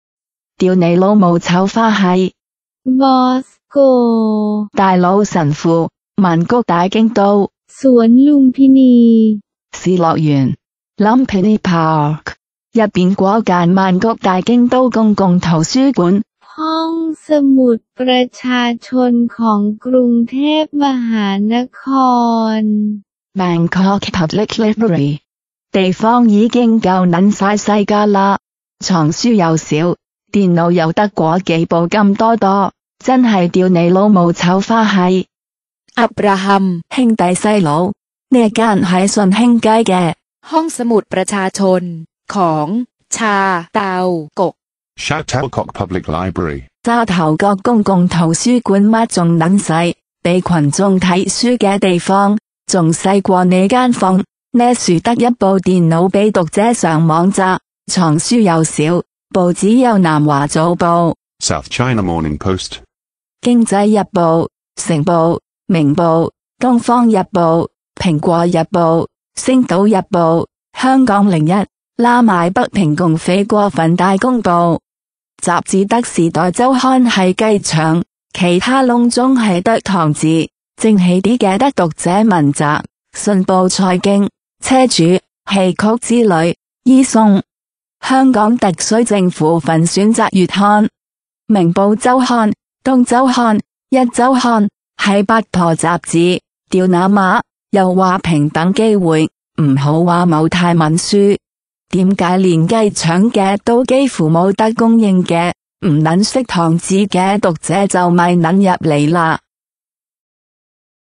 ห้องสมุดประชาชน ของ ชาเตากก (Sha Tau Kok Public Library (沙頭角公共圖書館))
Sha Tau Kok Public Library (ห้องสมุดประชาชน ของ ชาเตากก (沙頭角公共圖書館)) is located in Shun Hing Street (順興街), Sha Tau Kok (沙頭角), New Territories (新界).

沙頭角公共圖書館（Sha Tau Kok Public Library (ห้องสมุดประชาชน ของ ชาเตากก)）位於新界（New Territories）沙頭角（Sha Tau Kok）順興街（Shun Hing Street）。

Its actual reading area with few rubbish books, is even smaller than your sleeping room (800 square feet (74 square metres)), it just offers a computer for internet! It just offers eight newspapers, such as "South China Morning Post (南華早報)", "Economic Daily (經濟日報)", "Sing Pao Daily News (成報)", "Ming Pao (明報)", "Oriental Daily (東方日報)", "Apple Daily (蘋果日報)", "Sing Tao Daily (星島日報)", "HK01 (香港01)", and "Ta Kung Pao (大公報)", which is the propaganda tool of the evil Peking communist bandits; the "Time magazine (時代周刊)" is unique English magazine be available in here, Chinese characters is dominant in its bookshelves, such as "Reader's Digest (讀者文摘)", "Hong Kong Economic Journal Monthly (信報財經)", "Automobile Magazine Hong Kong (車主)", "Journey To Chinese Opera & Drama (戲曲之旅)", "e-zone", "Choice (選擇)" is published by the Consumer Council, Hong Kong government, and three gossip magazines: "Ming Pao Weekly (明報周刊)", "East Week (東周刊)" and "Next Magazine (壹周刊)".

其實際閱覽面積幾乎少於汝之臥室（800平方英尺（74平方米）），藏書極少！僅提供一台電腦上網！提供《南華早報（South China Morning Post）》、《經濟日報（Economic Daily）》、《成報（Sing Pao Daily News）》、《明報（Ming Pao）》、《東方日報（Oriental Daily）》、《蘋果日報（Apple Daily）》、《星島日報（Sing Tao Daily）》、《香港01（HK01）》，及北平共匪之《大公報（Ta Kung Pao）》等八份報紙（eight newspapers）；《（HK01）》；《時代周刊（Time magazine）》乃其惟一供應的英文雜誌（English magazine），其它雜誌盡皆中文，諸如《讀者文摘（Reader's Digest）》、《信報財經（Hong Kong Economic Journal Monthly）》、《車主（Automobile Magazine Hong Kong）》、《戲曲之旅（Journey To Chinese Opera & Drama）》、《e-zone》，消費者委員會《選擇（Choice）》，以及三份八封雜誌（three gossip magazines）：《明報周刊（Ming Pao Weekly）》、《東周刊（East Week）》、《壹周刊（Next Magazine））》。

If you are not familiar with Chinese characters, you better keep clear of this library!
如果汝等不熟悉中文，汝等最好不要蒞臨此圖書館！

Opening Hours:
(i) 10:00 a.m. to 1:00 p.m. and 2:00 p.m. to 6:00 p.m. on Monday, Wednesday and Saturday;
(ii) 1:00 p.m. to 8:00 p.m. on Tuesday and Friday;
(iii) 9:00 a.m. to 1:00 p.m. on Sunday and some public holidays;
(iv) closed on every Thursdays, the first three days of Chinese lunar new year, Good Friday (Jesus Christ's death at Calvary), Christmas and British boxing day (the day after Christmas day).